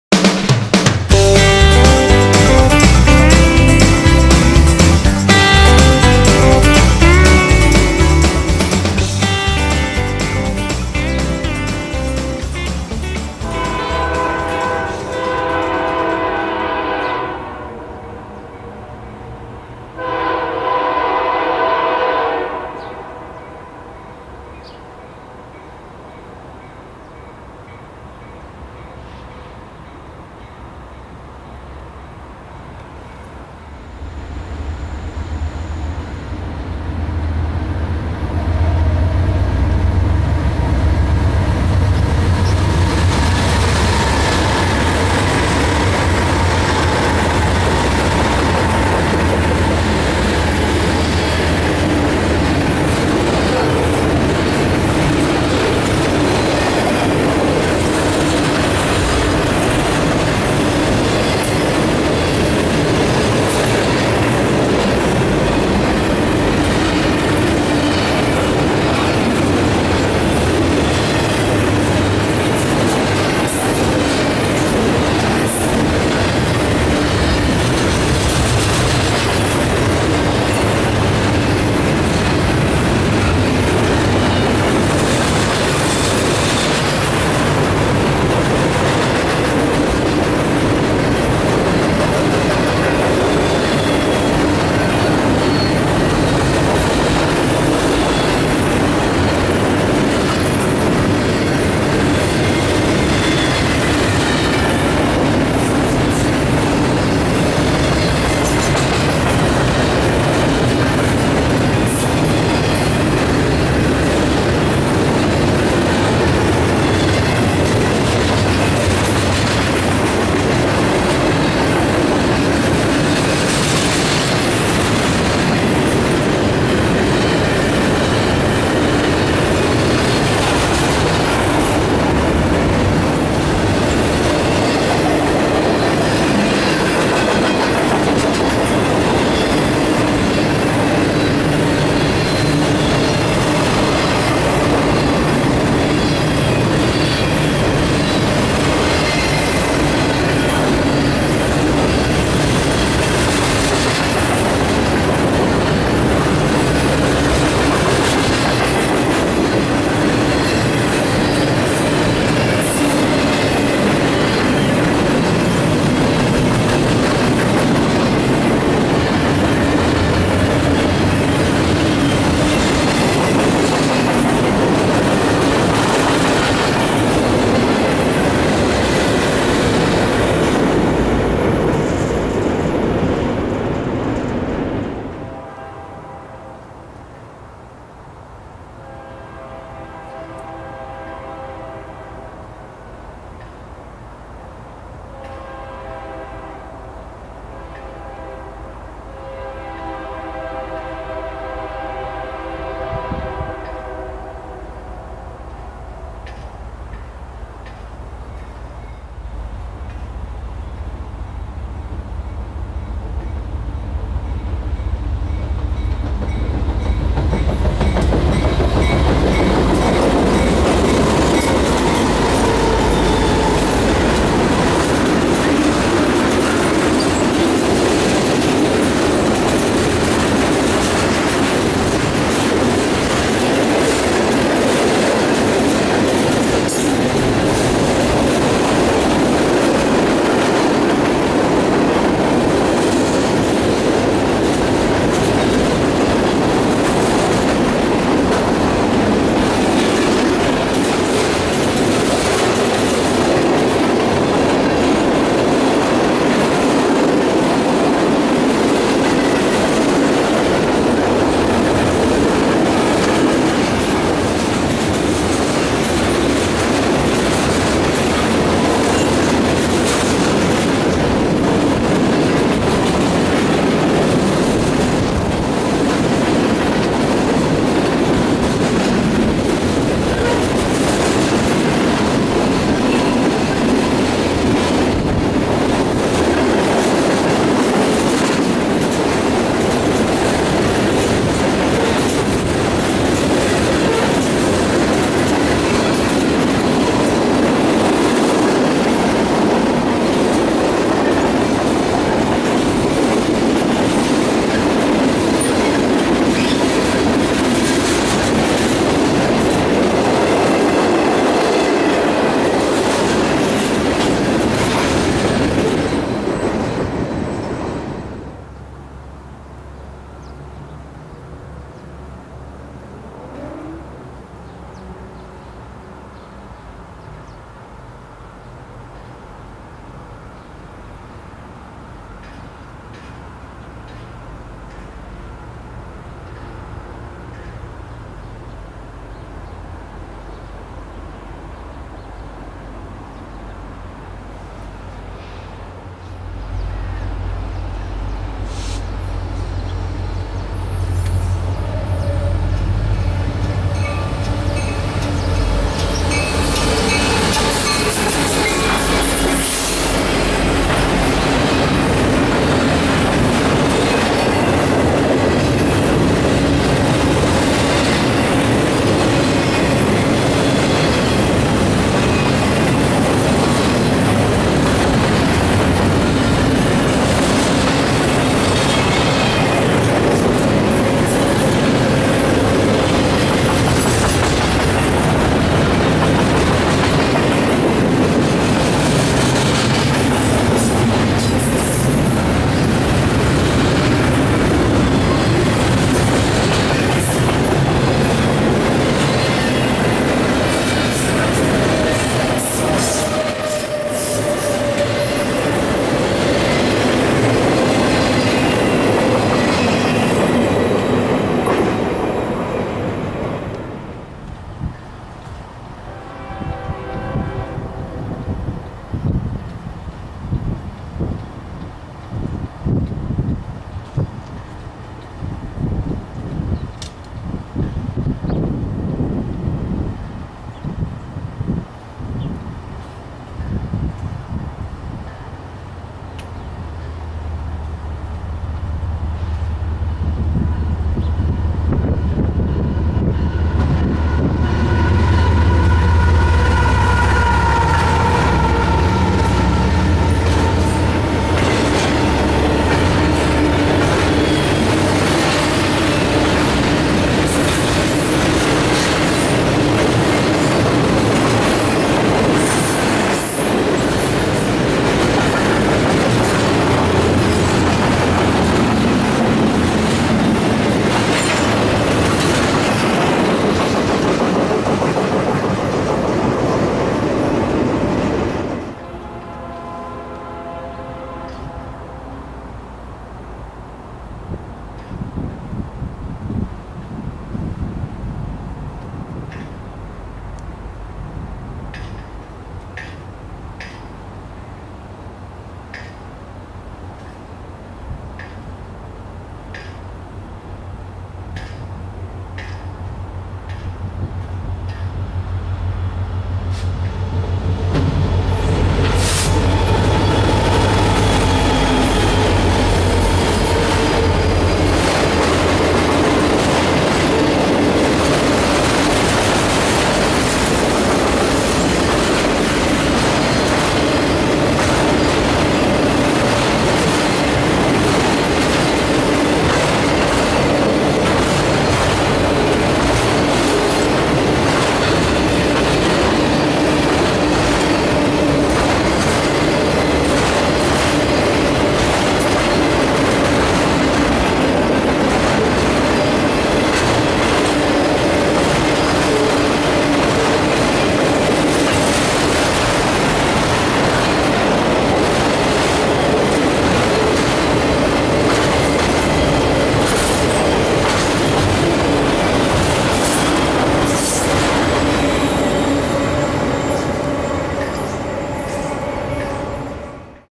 Early Morning. Late Evening
This episode is based on what I saw in the early morning hours before I took Amtrak #501 to Portland for the day & returning in the late evening on train #508.